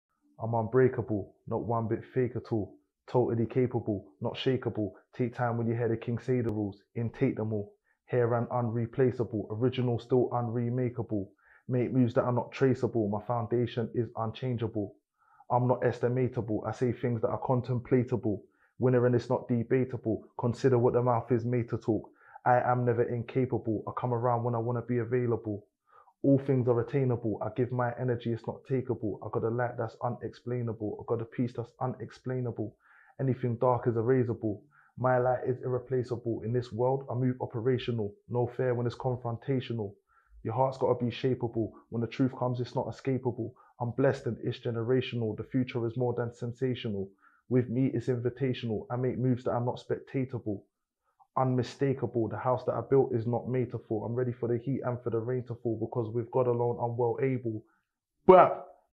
Novelist x Lewisham Music
Lewisham legend, Novelist swung by our new community spaces in Bellingham to check out the aptly named Novelist Studio, and couldn't resist spitting a few bars and sharing some words of wisdom for the young people.